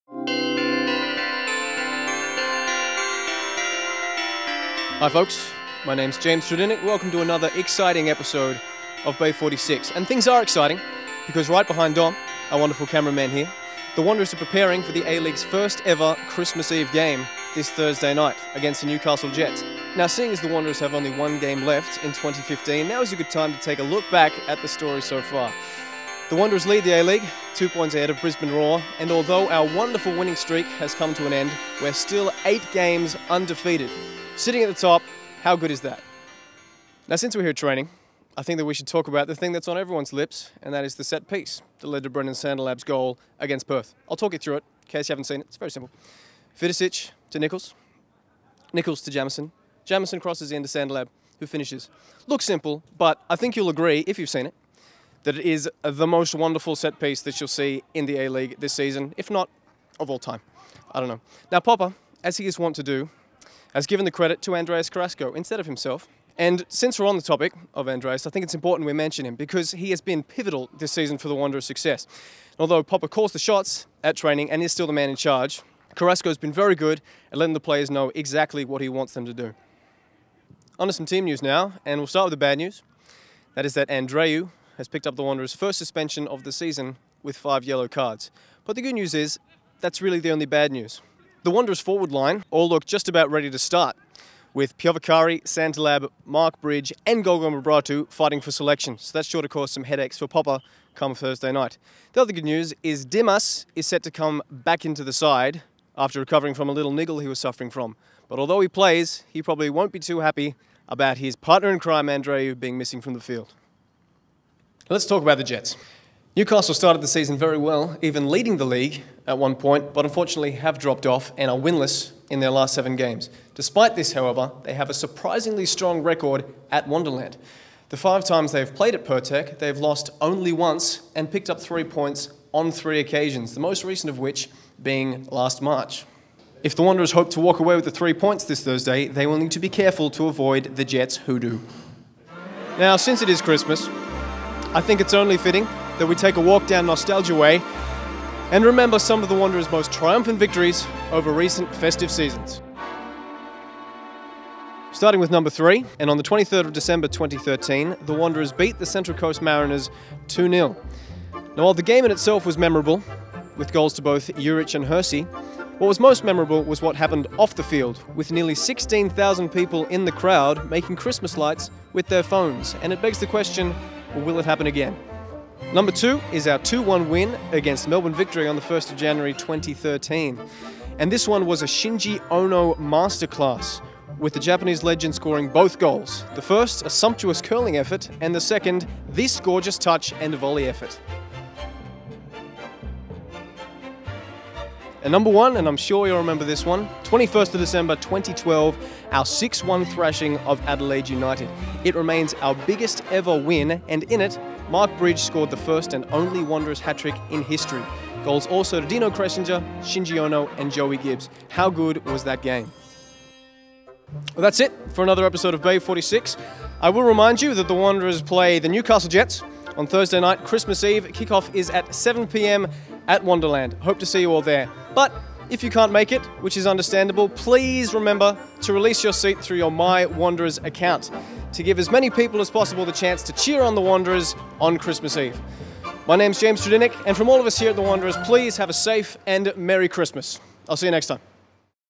Bay 46 | Christmas Eve lights up Wanderland
James popped by the stadium to see how the boys were preparing for Christmas Eve at Wanderland.